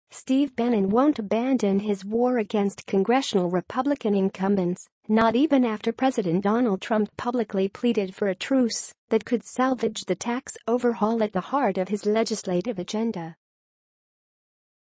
Trump’s Bid for Bannon Truce Signals Tax Plan Fears
Steve Bannon won’t abandon his war against congressional Republican incumbents, not even after President Donald Trump publicly pleaded for a truce that could salvage the tax overhaul at the heart of his legislative agenda.